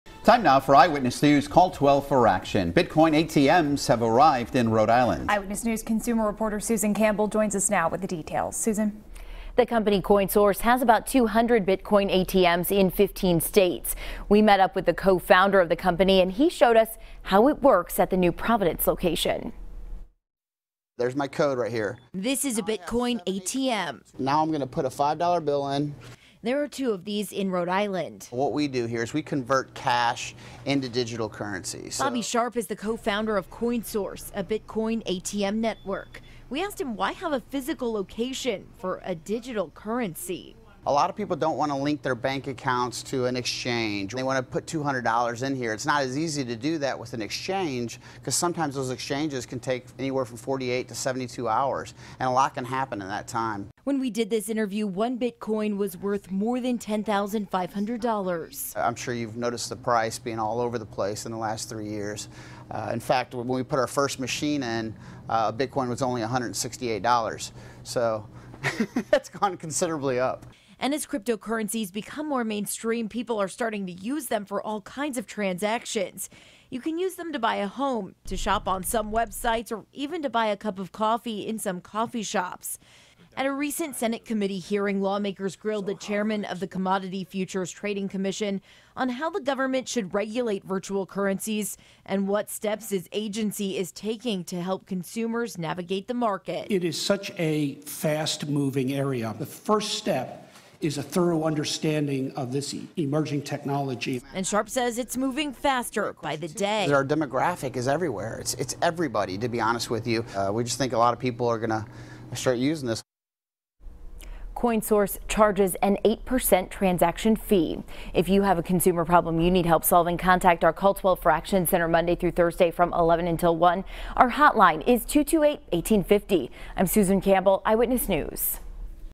Bitcoin ATMs have arrived in Rhode Island
“What we do here is we convert cash into digital currency,” Coinsource co-founder Bobby Sharp said.